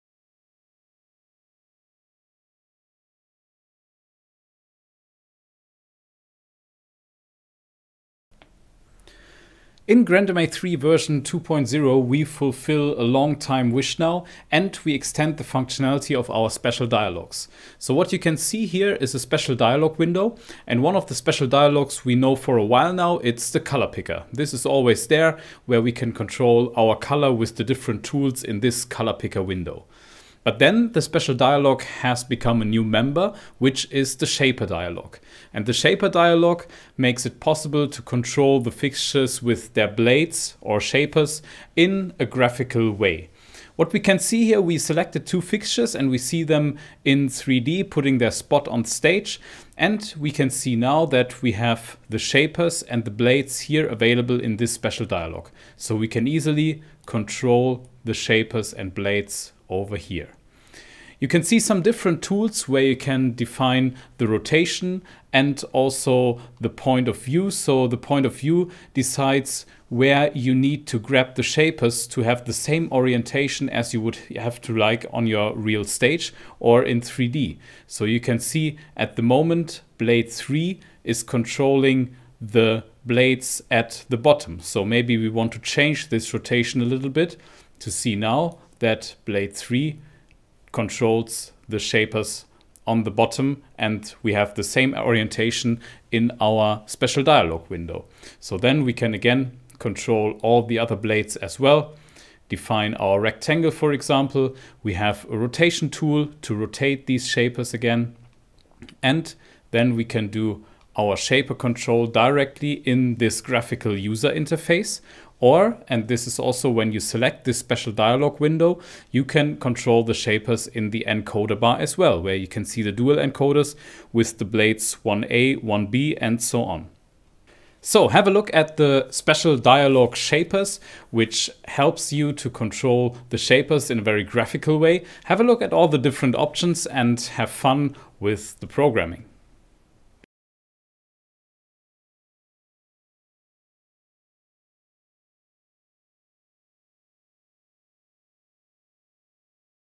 grandMA3 | Software Release Version 2.0 | Special Dialogs
Have a look at the next chapter of Special Dialogs. It will help you to have fast and easy control of your shapers!